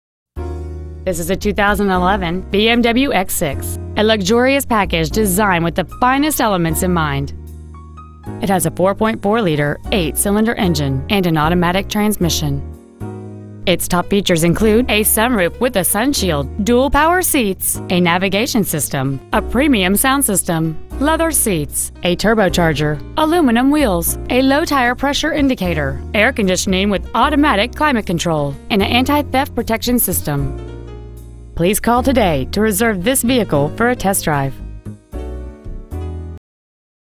2011 BMW X6 IN
Year : 2011
 Make : BMW
 Model : X6
 Engine : 4.4L V8
 Trans . : Automatic
 Exterior : Blue

 Interior : Black
 BMW of Schererville
 1400 U.S. 41
  , IN 46375